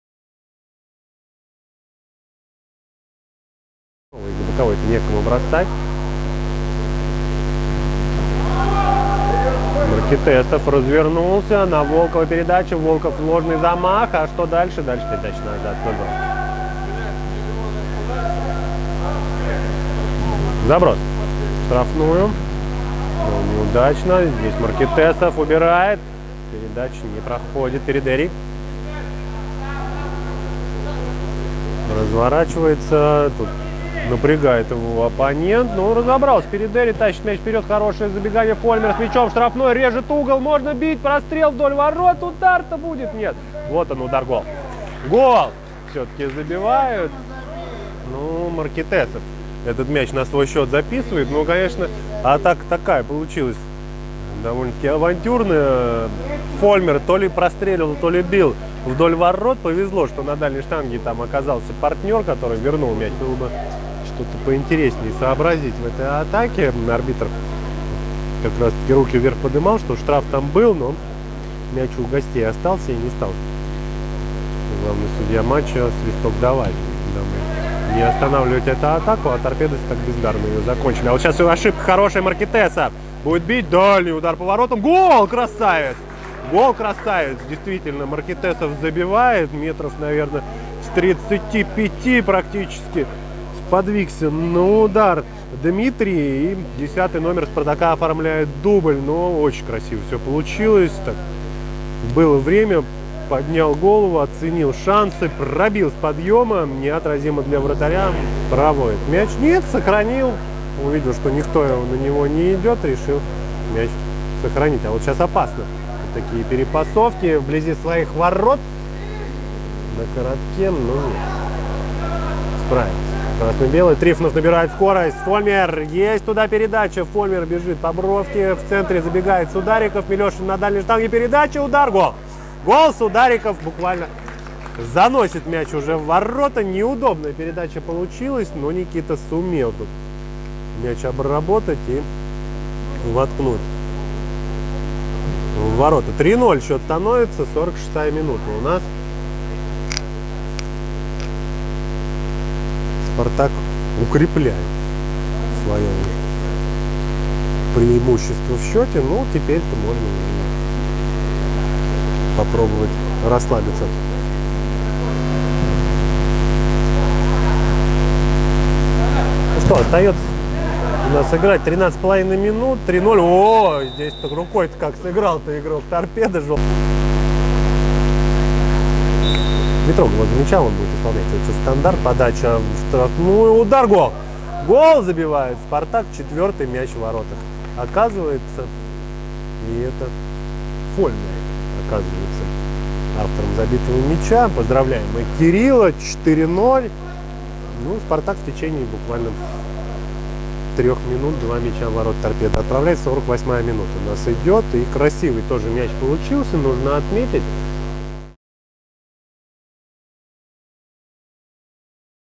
"Спартак" (2000 г. р.) - "Торпедо" 4:0
Зимнее первенство Москвы / Клубная лига / 3-й тур
«Спартак» — «Торпедо» (команды 2000 г. р.) — 4:0 (2:0)
Голы: Маркитесов — 2, Судариков, Фольмер.